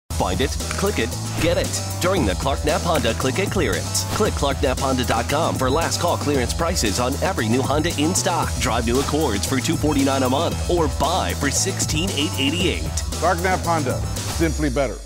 Clark Knapp Honda - Click It Clearance Accord
- Come on down for the Click It Clearance Event at Clark Knapp Honda and take advantage of sales on the 2018 Accord and more!